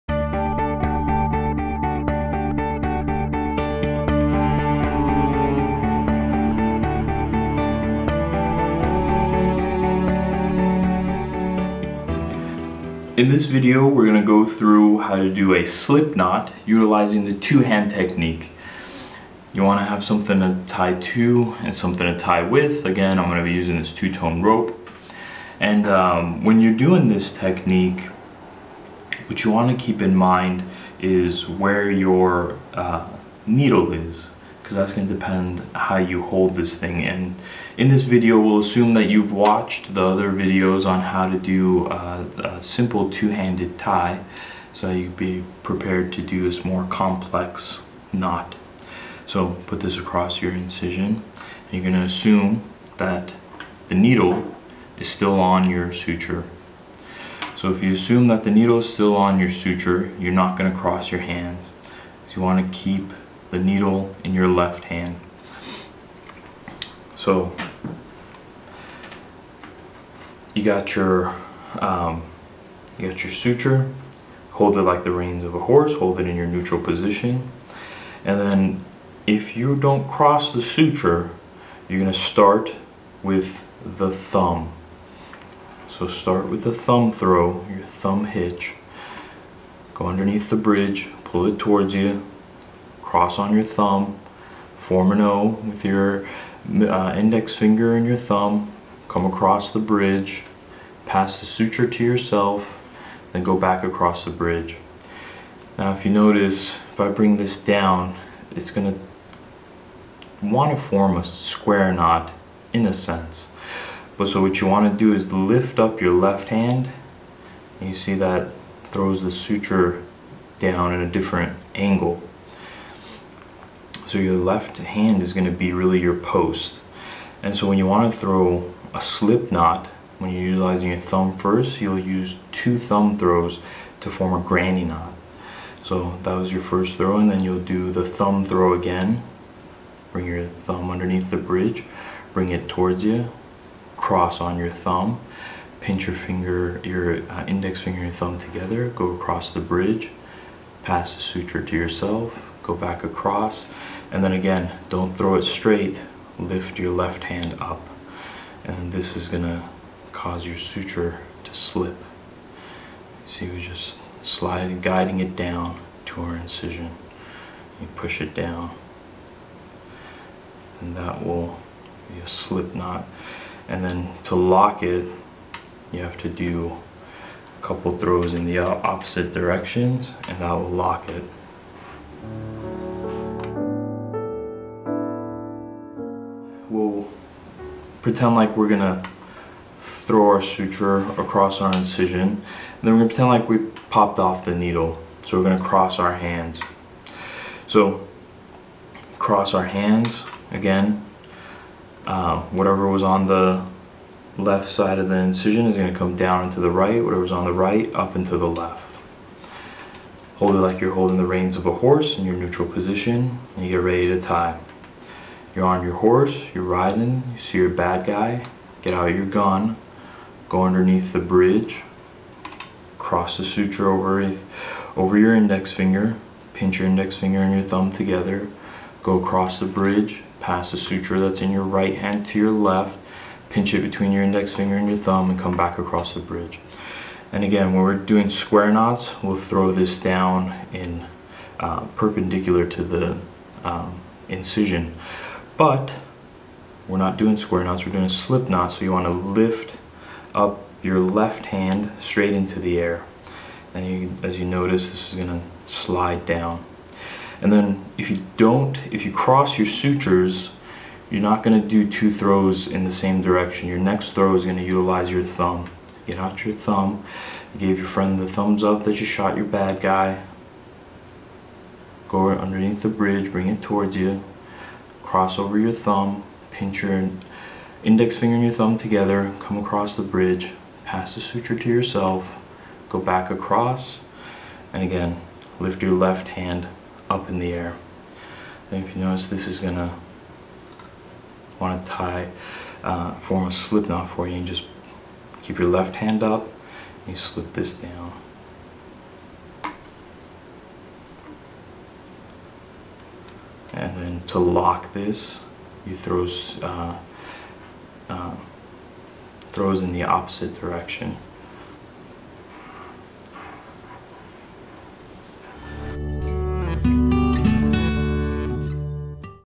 BEST demonstration of how to form a surgical slip knot; Two hand technique
How to video on tying surgical knots. Become a pro in the operating room by mastering knot tying techniques. Step by step instruction by Dr. Lalezari as part of a larger series of videos on surgical technique on how to perform a surgical knot utilizing a slip knot with the two hand technique.

These knots are used when trying to reapproximate tissue that is under tension.

Narrated by Dr. Lalezari